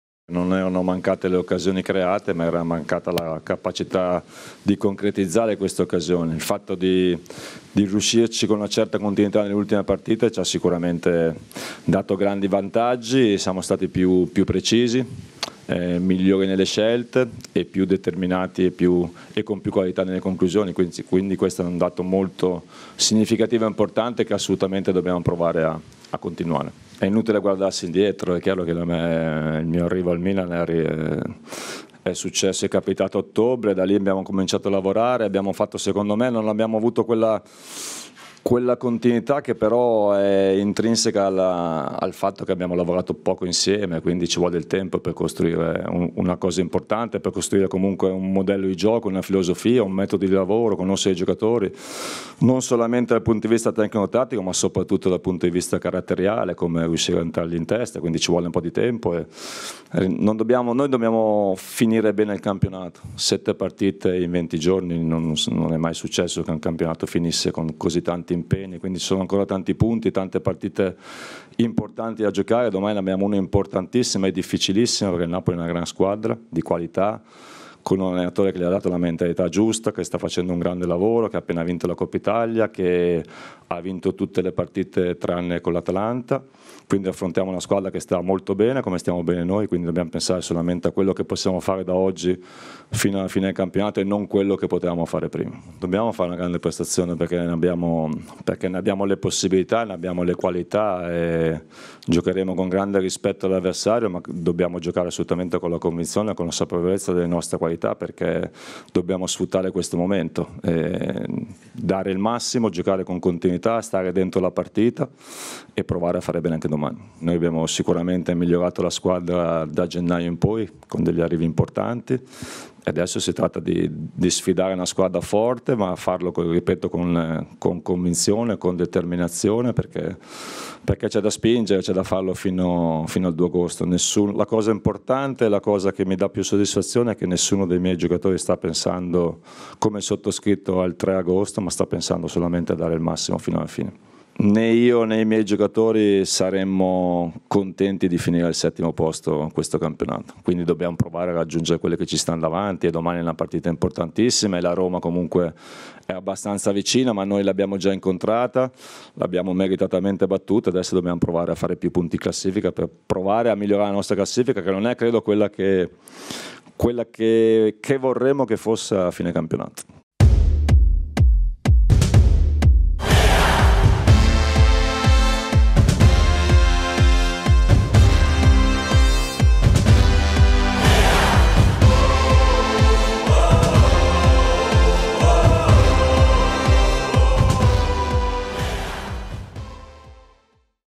Pre-match Press Conference
The Coach's press briefing ahead of our trip to Napoli